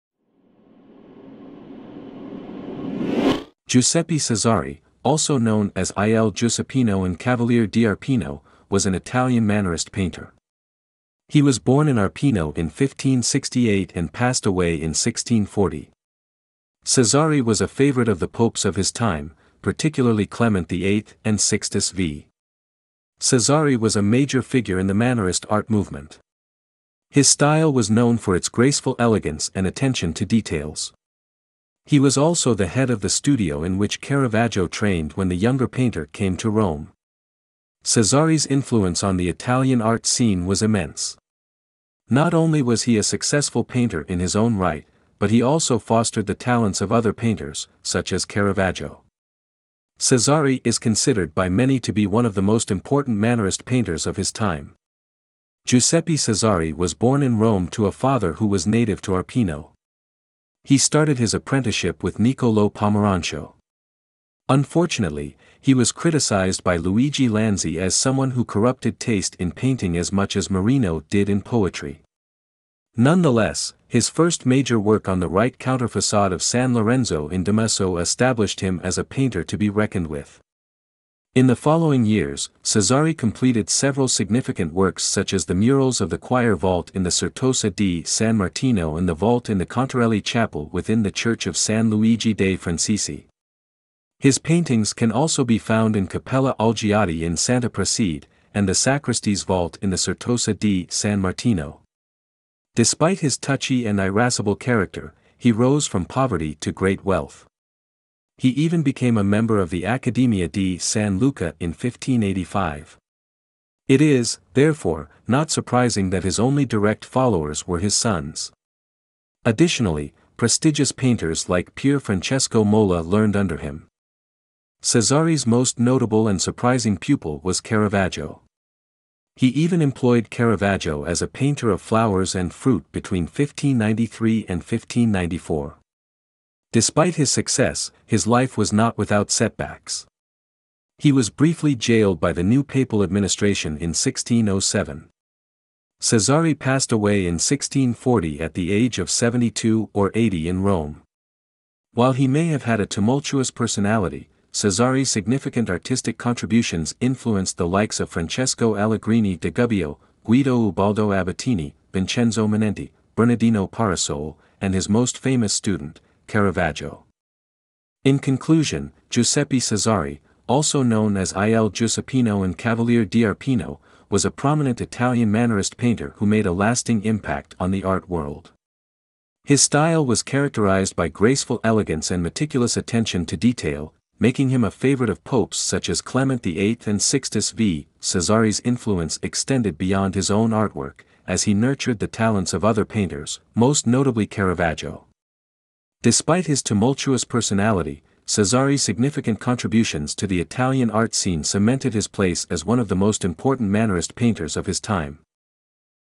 Who is Giuseppe Cesari｜Artist Biography｜VISART
Giuseppe Cesari, also known as Il Giuseppino and Cavaliere d'Arpino, was an Italian Mannerist painter.……

A biography of Giuseppe Cesari.